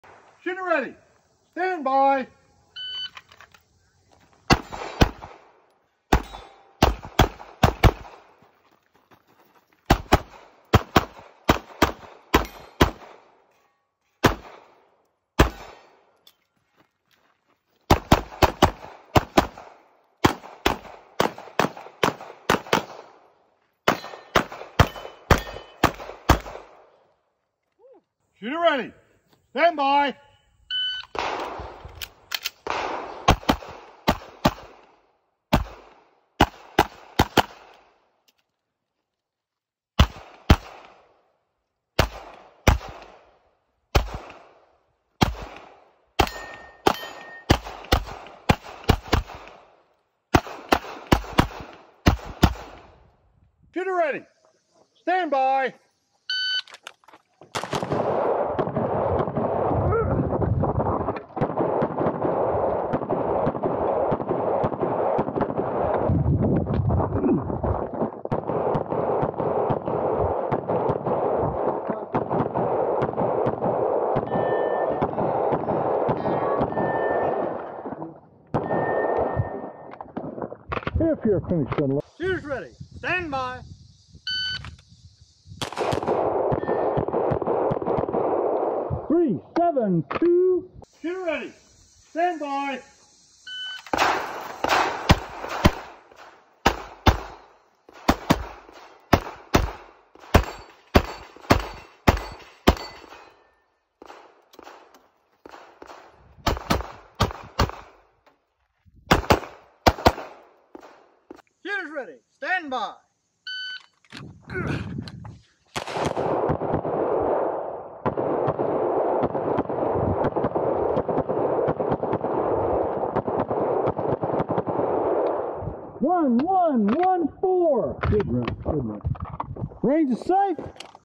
WVRPC USPSA September 2, 2023
2nd in Carry Optics and beat the A and B class guys despite of me being impatient on the steel on my 1st stage and some steel calibration issues. Also had my 1st A class classifier run at the matches and 3 stage wins!